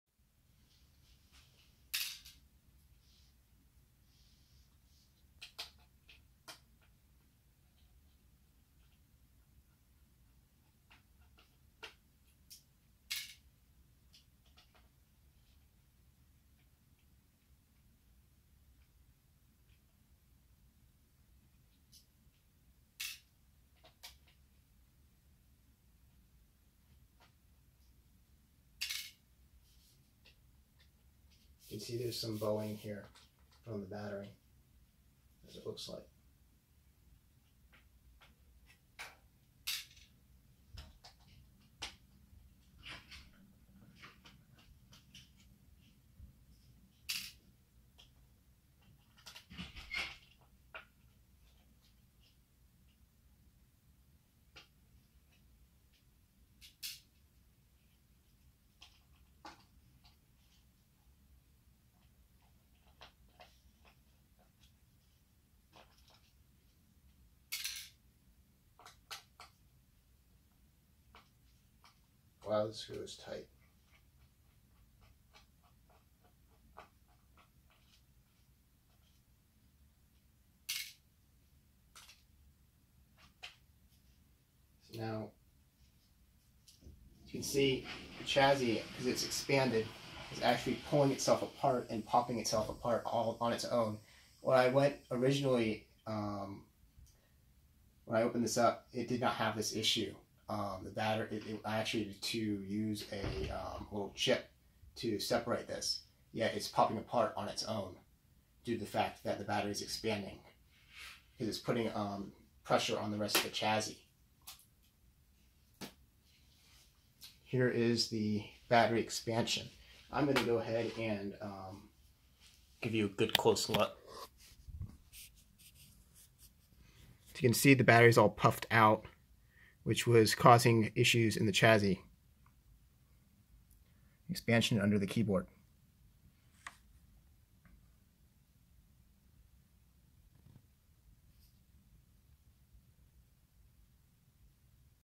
GX501 Battery Expansion Emergency Disassembly
Battery Expanision in a solid metal chassis like the GX501 poses a serious risk for fire or explosion.
The expansion in this GX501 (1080 MaxQ) was so bad it distended the keyboard and was popping keycaps.